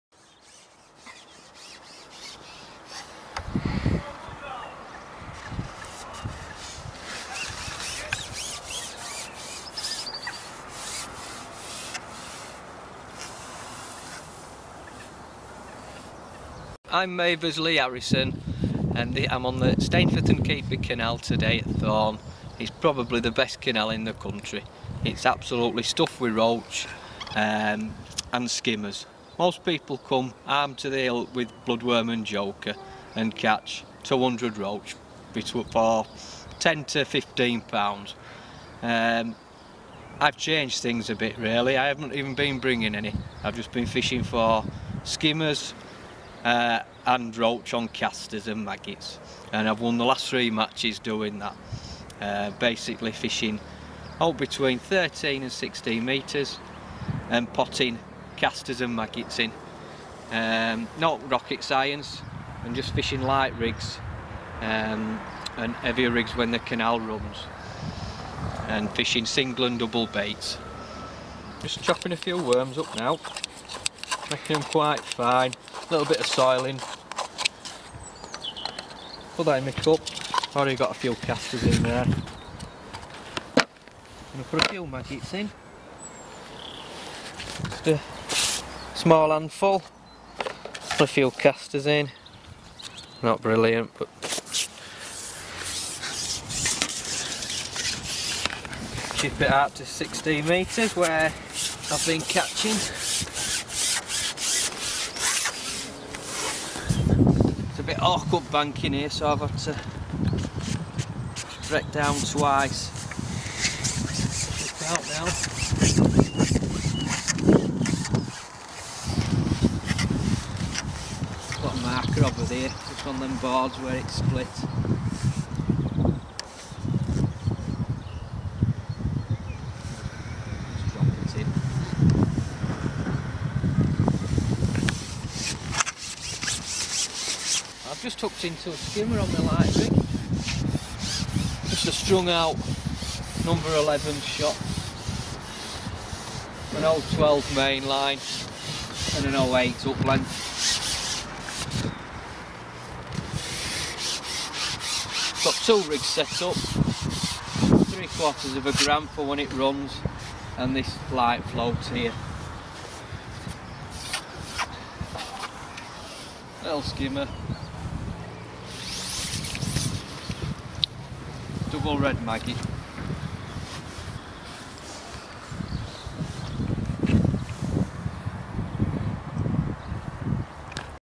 Lee Harrison on Stainforth and Keadby Canal
Maver-backed match angler Lee Harrison shows how he fishes for the skimmer bream in the Stainforth and Keadby canal at Thorne, In my opinion as well as Lee's is the best canal in the country!